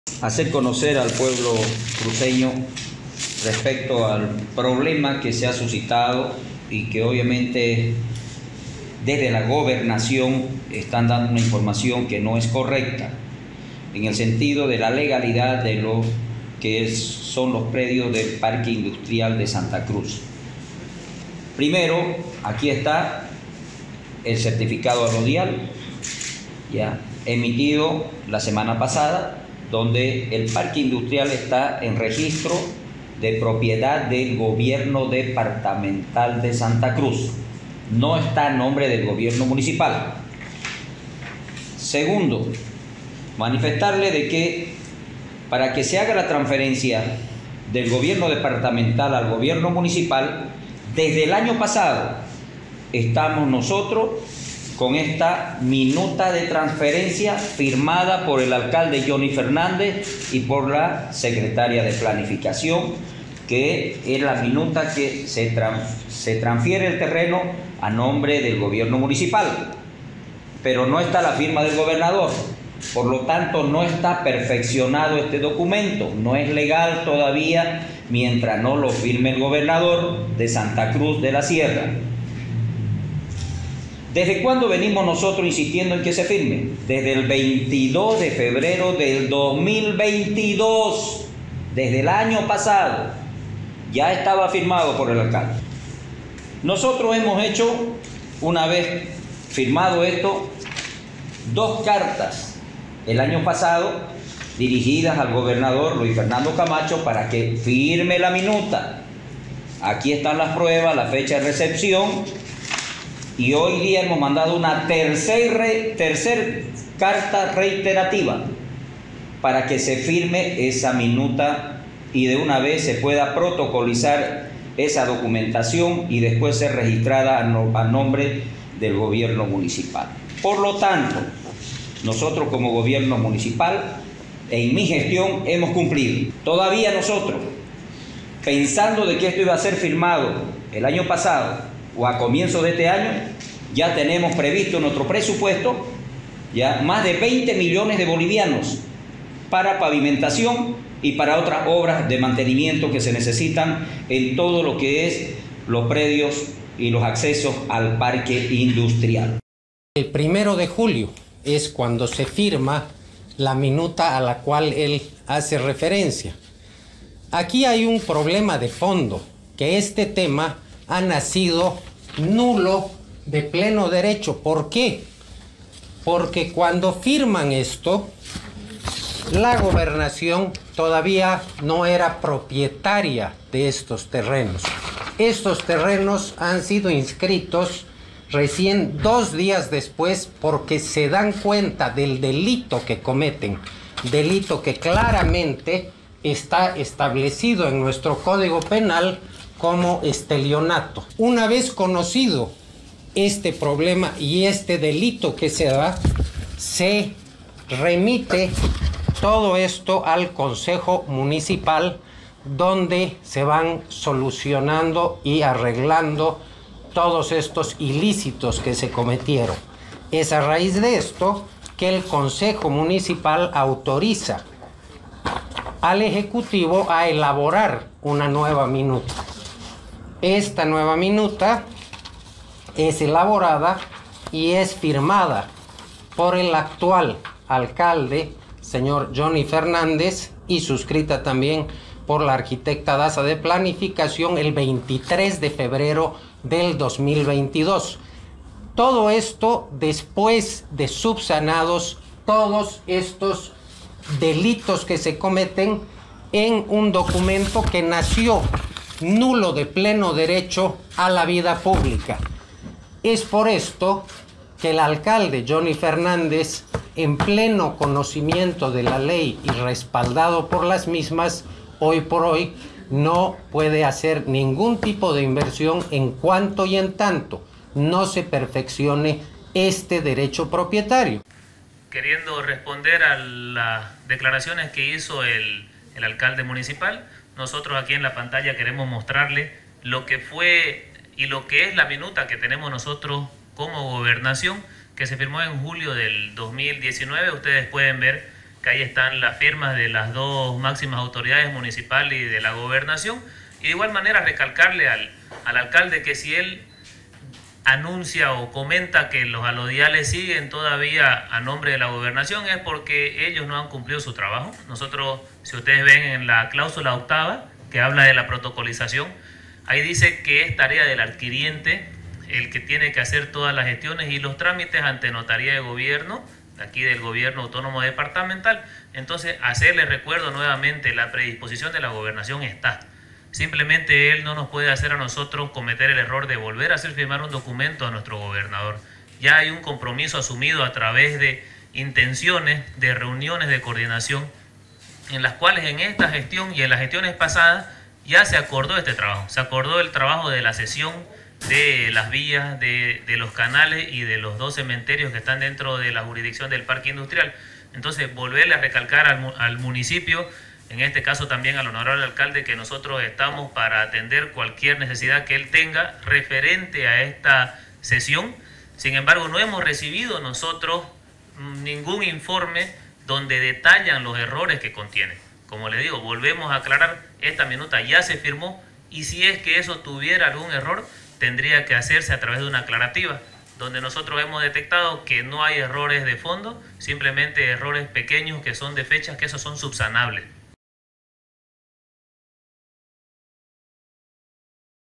Parque industrial enfrenta a Alcaldía y gobernación, cada uno asegura que la propiedad es del otro,
Parque industrial enfrenta a Alcaldía y gobernación, cada uno asegura que la propiedad es del otro, obras se hacen esperar